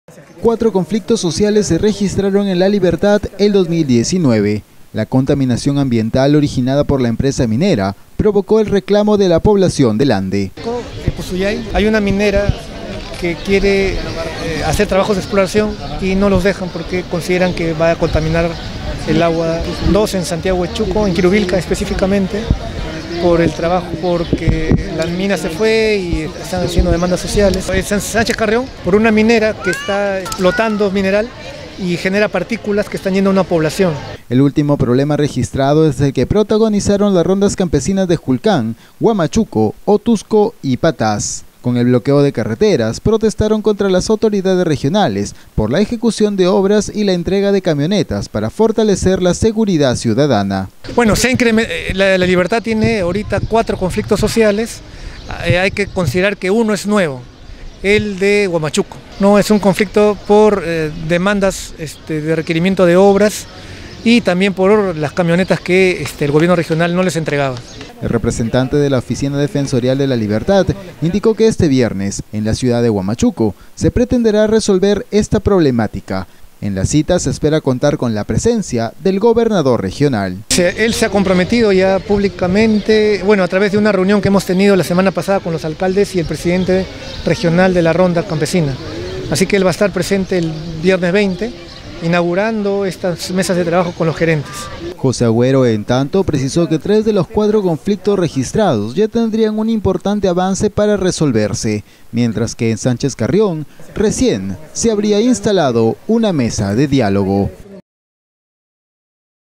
La Libertad registra cuatro conflictos sociales en lo que va del 2019
El representante de la Oficina Defensorial La Libertad informó que durante el año se registraron cuatro conflictos sociales, la contaminación ambiental y la minería provocó el reclamo de la población, entre estas problemáticas también fue considerada la última protesta realizada por las rondas campesinas de Huamachuco contra el Gobierno regional.